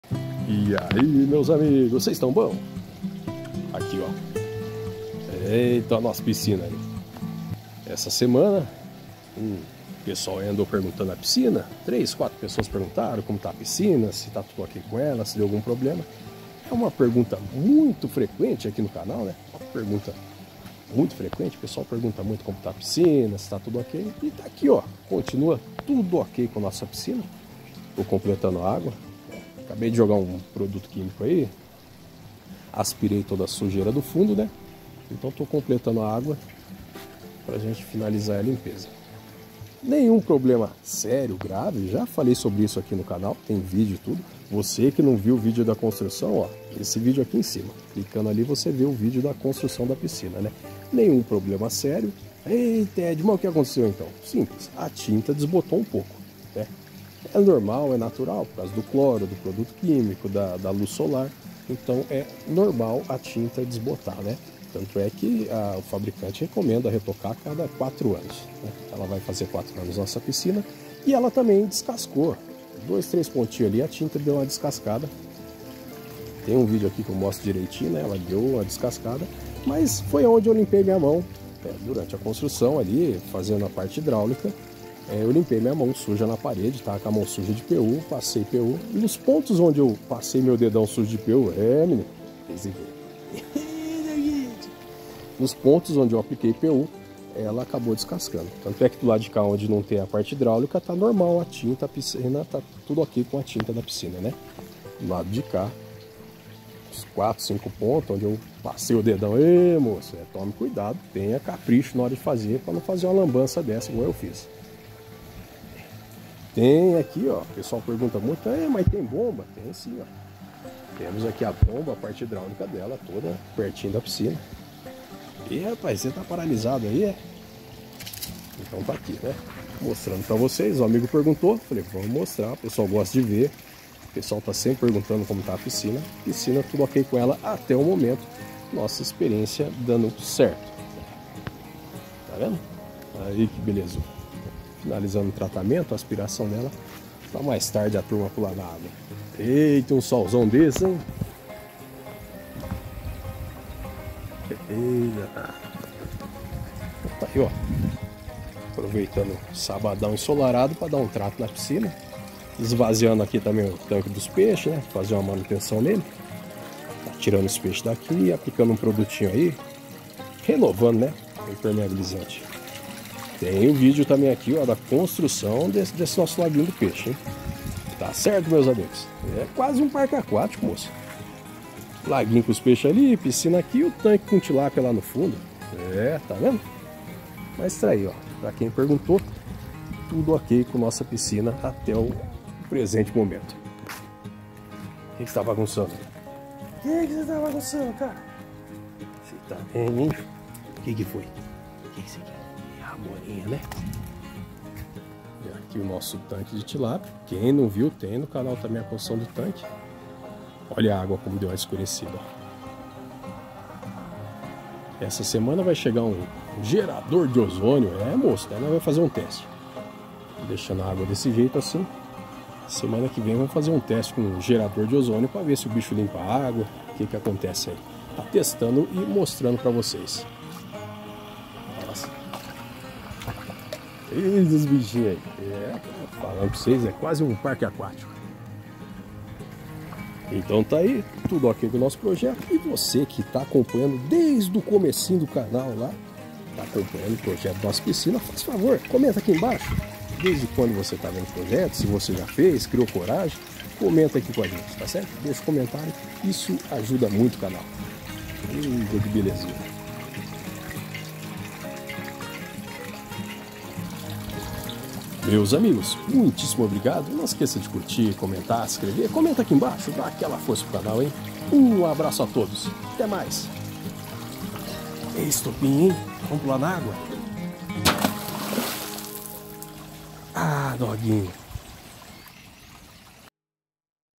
A pergunta que mais recebo no canal
Basta fazer calor que recebo varias mensagens perguntado sobre a piscina,
e podem continuar perguntando que faço questão de responder.